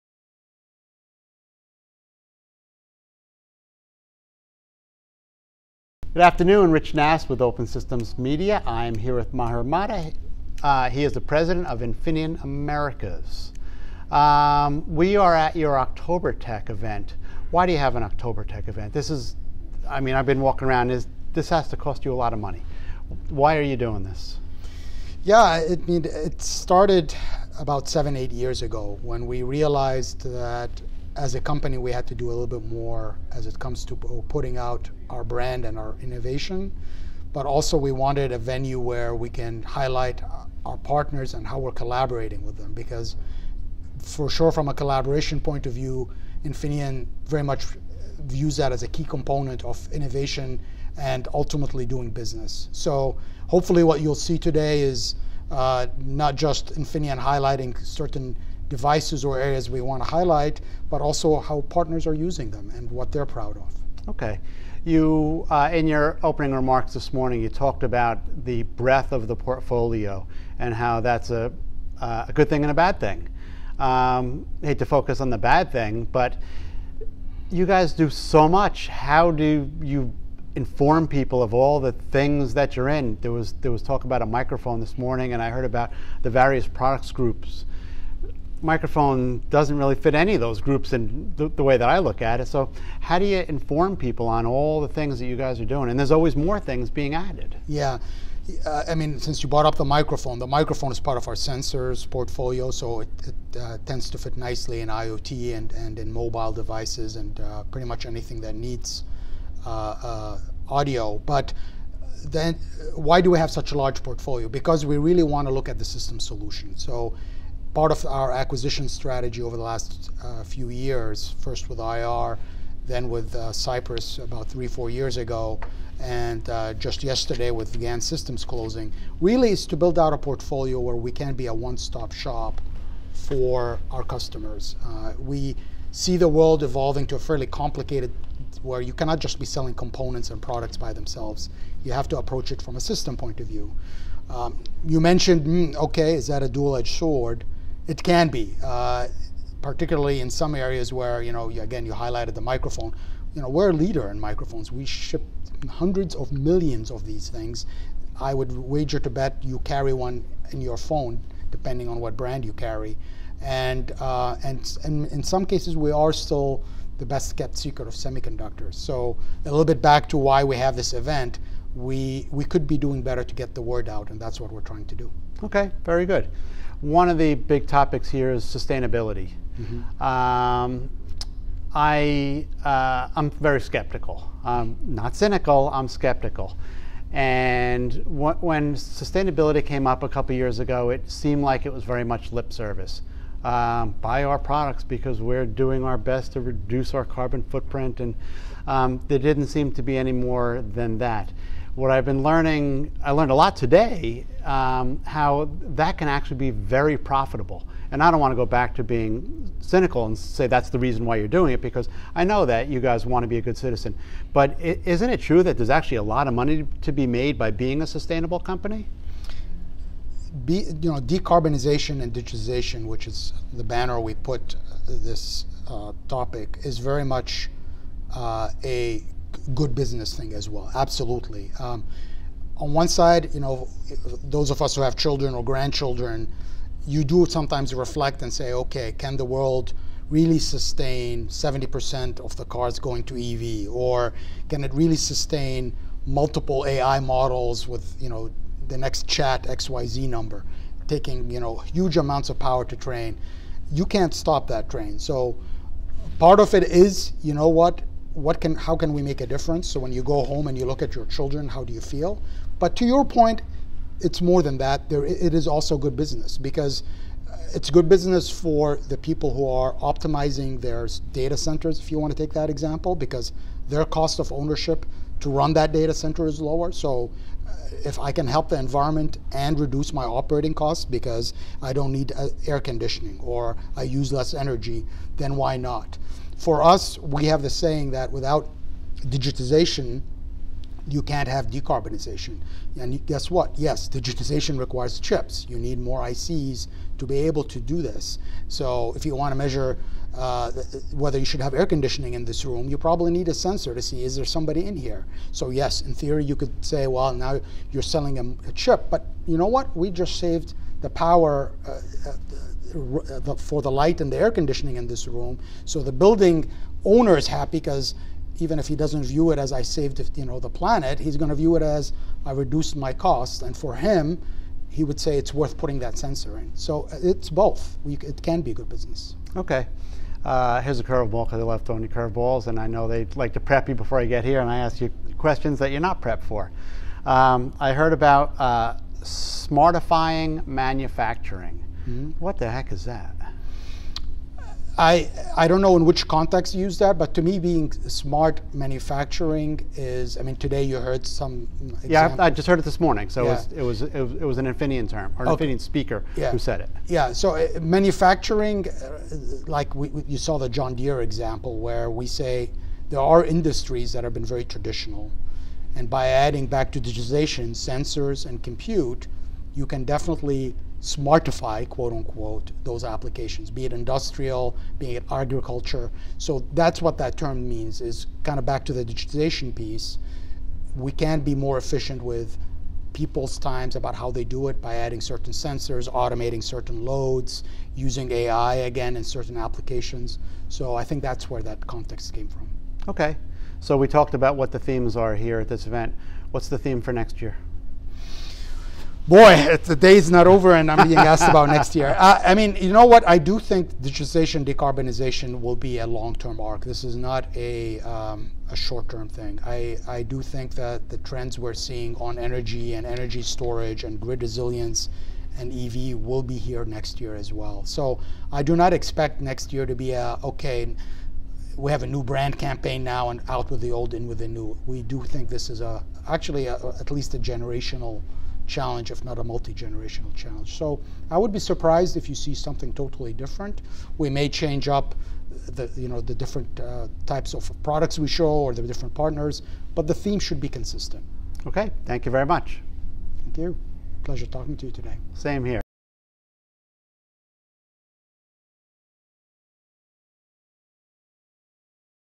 Embedded Executive Video: Maher Matta
Infineon recently held its annual OktoberTech event, where it showcased the company’s vast array of technologies as well as those of its customers. Embedded Computing Design’s Rich Nass sat down with Infineon Americas President Maher Matta to discuss things like sustainability, how to deal with such a wide range of products, and the need for an event like OktoberTech.